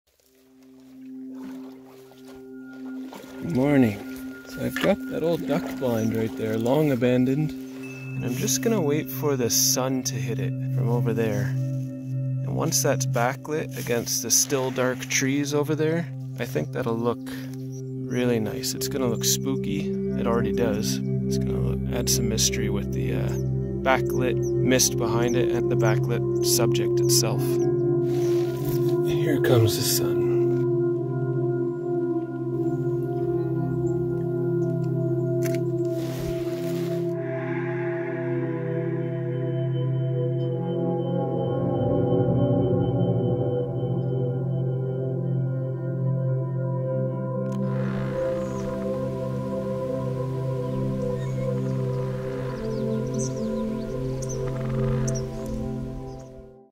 Creepy Photography - Duck Blind from Canoe
Come along for a quick paddle in the canoe as I photograph this creepy structure out in the sticks! The abandoned duck blind sits on stilts in a lake in North Frontenac County, Ontario. I wait for the sun to backlight it just right from my canoe.